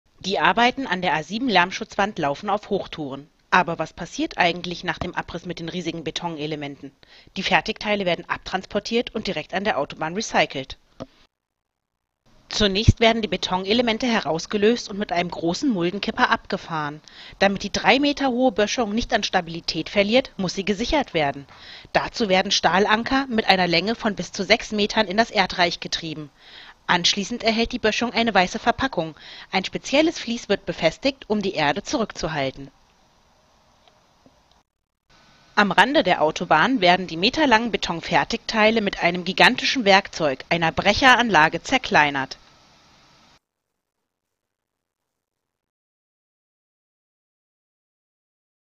A7: Lärmschutzwände werden recycelt
Kassel/Lohfelden, 13. Mai 2015: Arbeiten an der A7-Lärmschutzwand: Die Betonelemente der alten Lärmschutzwände werden direkt neben der Autobahn zu Recyclingmaterial verarbeitet.